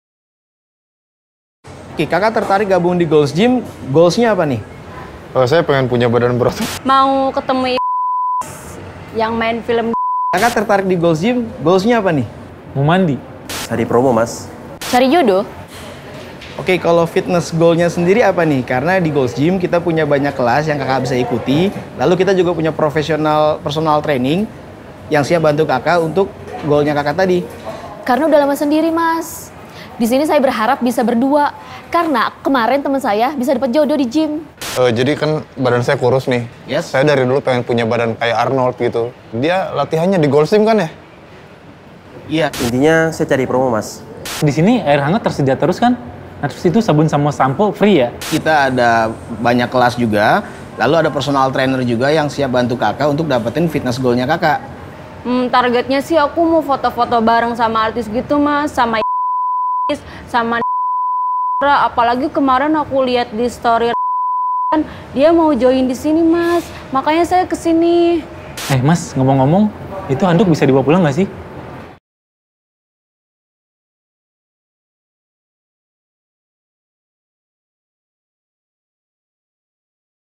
Why Join a Gym? Finding Love, Meeting Celebrities, and a Place to Take a Shower
A funny new campaign from Gold's Gym in Indonesia shows people on CCTV getting 'real' about their reason to join the gym.

Credits

Client: Gold’s Gym Indonesia
Agency: LUP Jakarta
Executive Creative Director: Albert Chan
Creative Director: Andika Nugroho
Art Director: Ardho Khalifa, Rahmi Amelia
Copywriter: Imam Yudia
Account Manager: Annisa Islamey

Uploaded with permission of LUP Jakarta